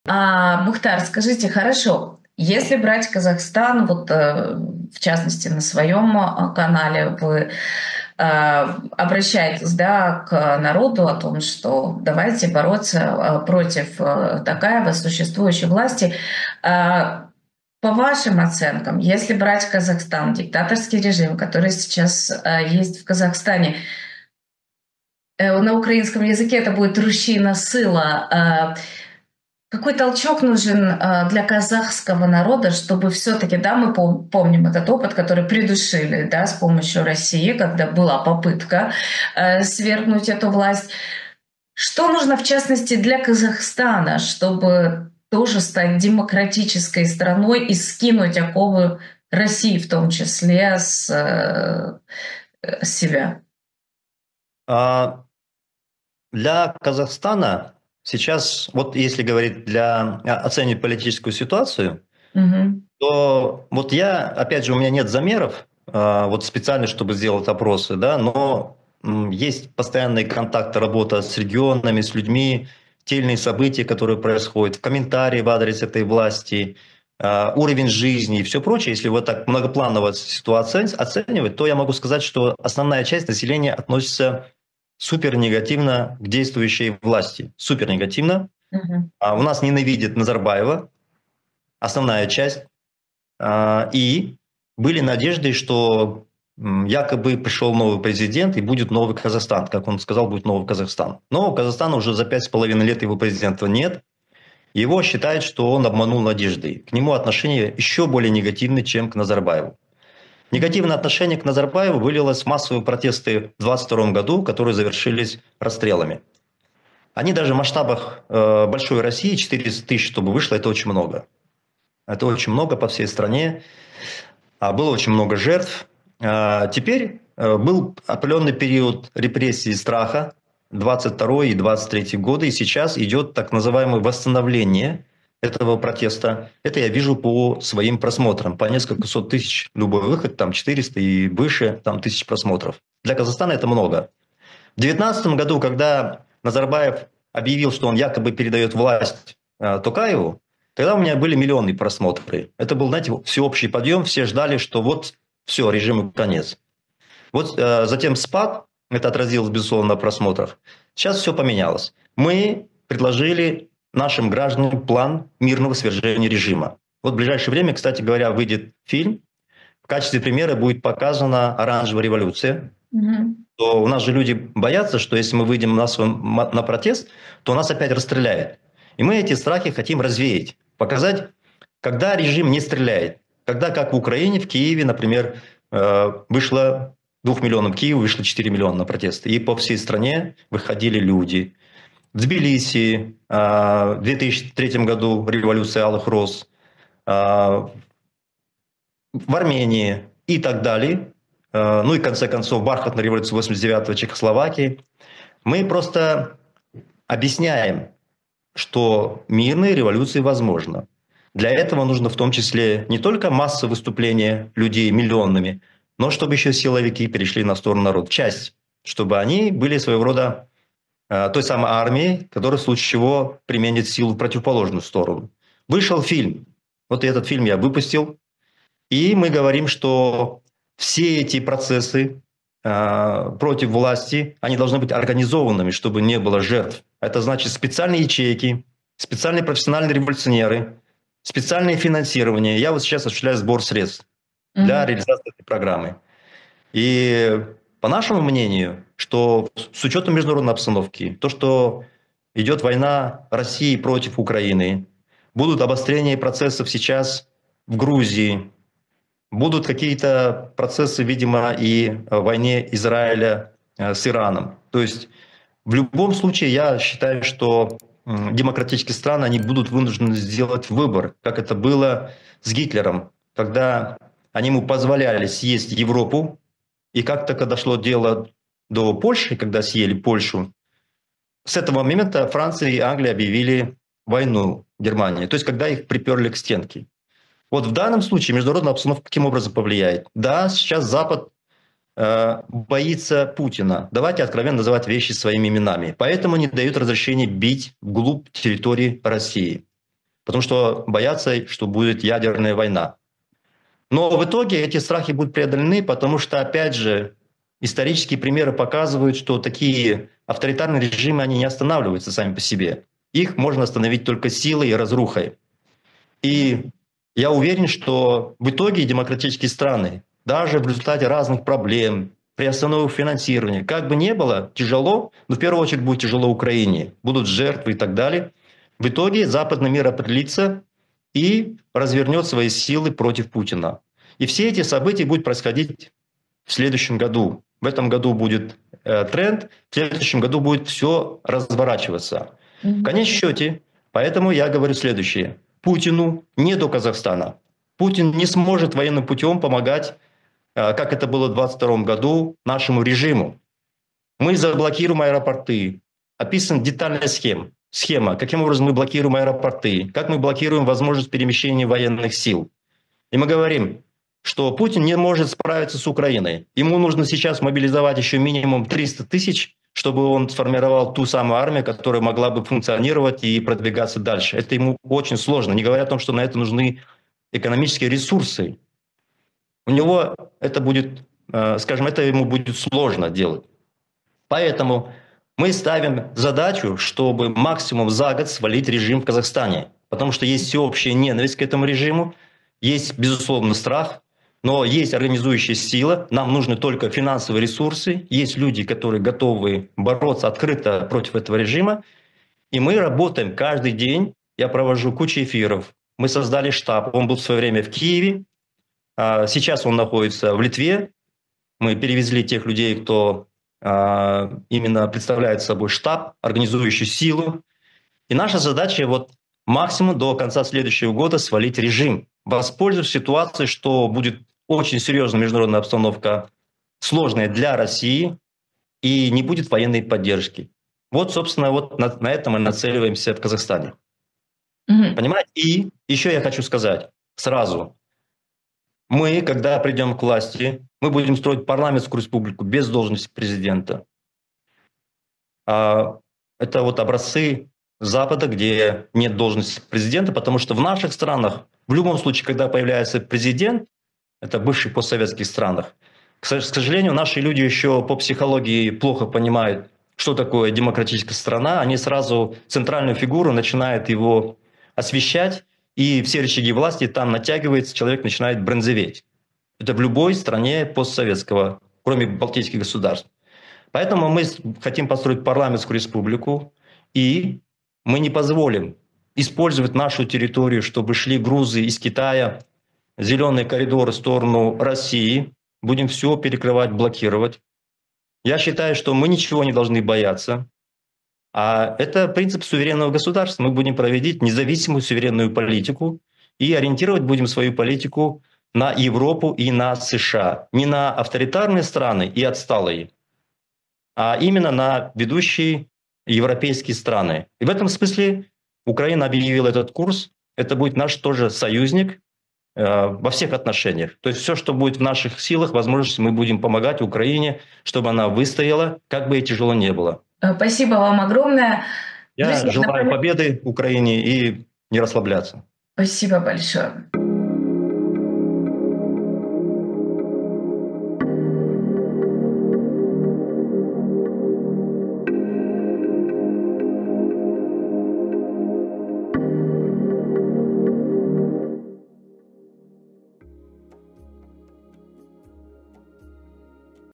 Восстание в Казахстане: режим Токаева свалят в 2025, и Путин ему не поможет
RU - Режим Токаева должен быть свален до конца следующего года, такую задачу ставит себе оппозиция Казахстана. И Путину будет не до того, чтобы помогать ему в сохранении власти. Подробнее тему анализирует Мухтар Аблязов, казахстанский оппозиционер.

UA - Режим Токаєва має бути звалений до кінця наступного року, таке завдання ставить собі опозиція Казахстану. І Путіну буде не до того, щоб допомагати йому у збереженні влади. Докладніше тему аналізує Мухтар Аблязов, казахстанський опозиціонер.